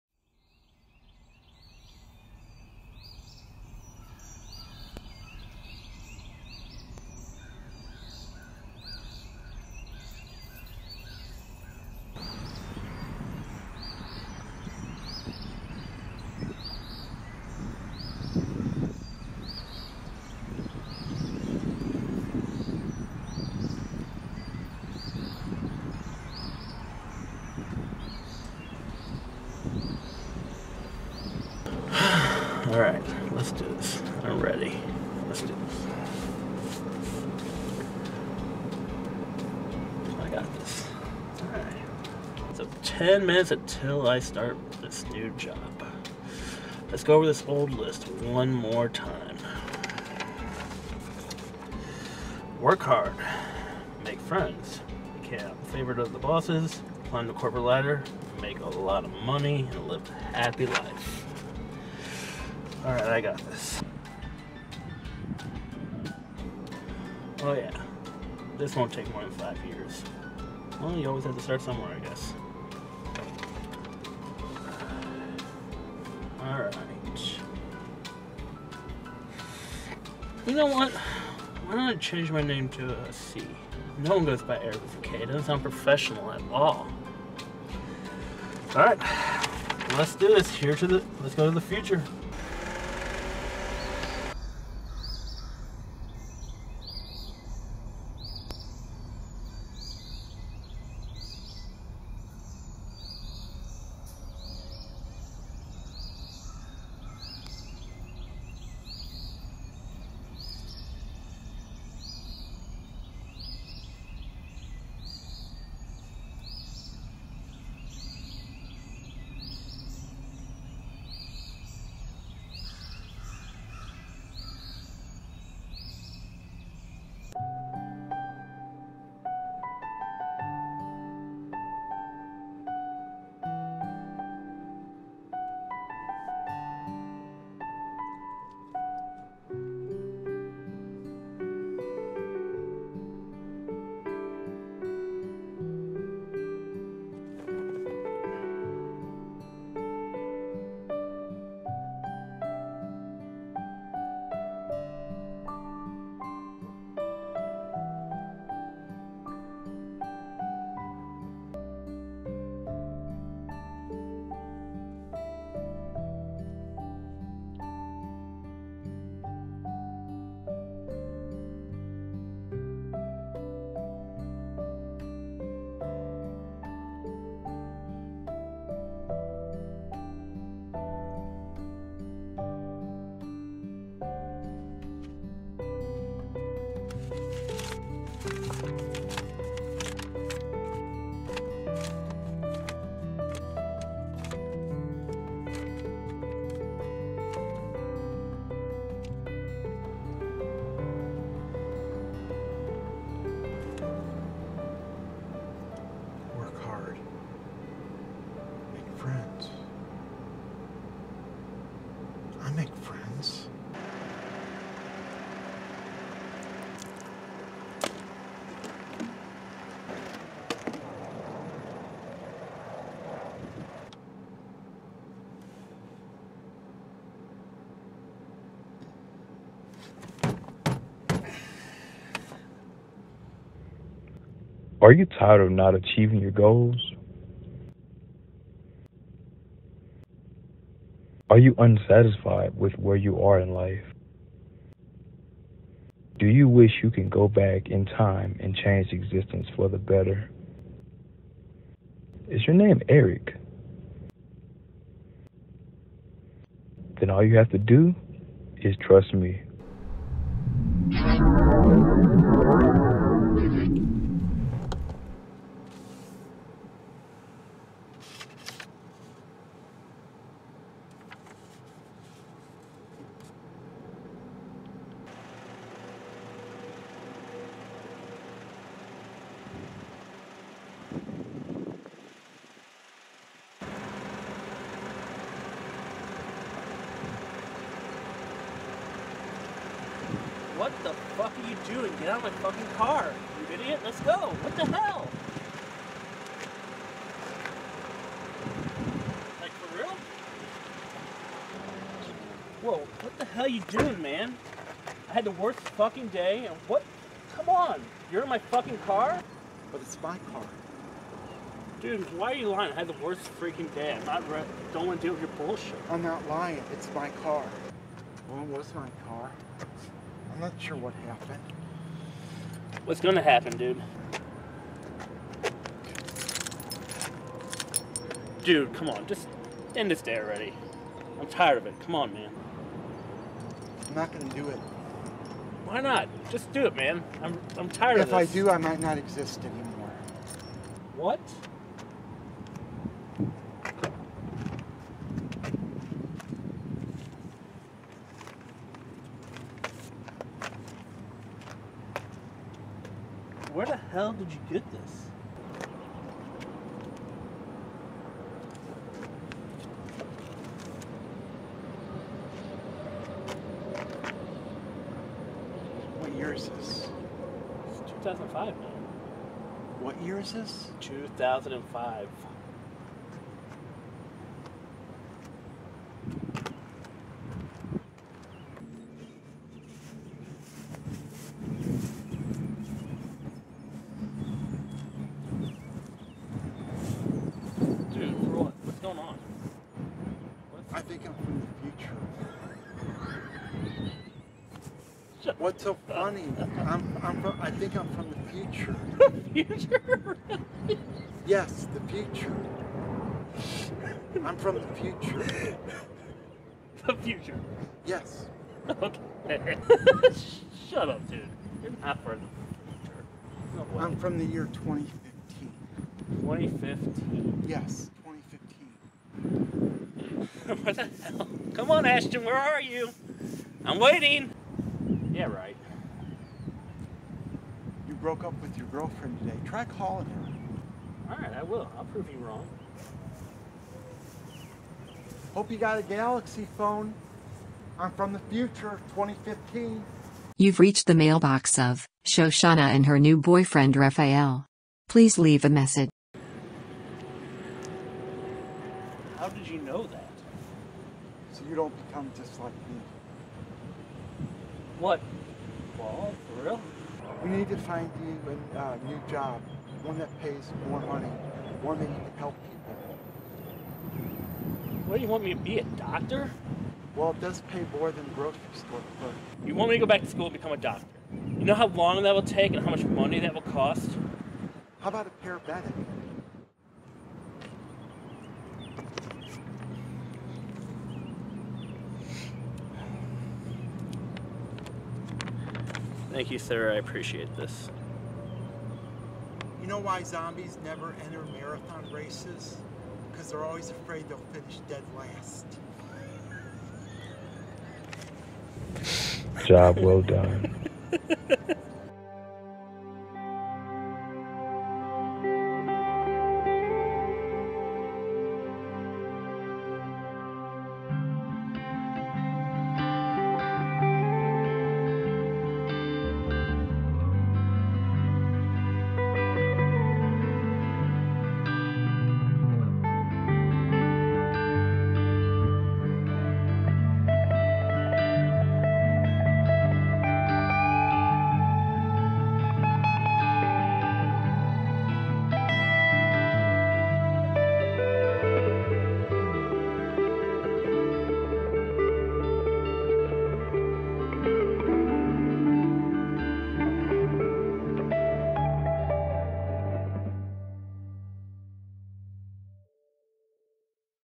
Eric Through the Ages
I've always wanted to make a time travel movie, and after two good friends suggested an interesting plot, I knew I had to make this video. All the driving force behind this project was thanks to Eric and Erik, and would not be possible without them. Enjoy!

P.S.
This video is also hinting at the cinematic universe I mentioned.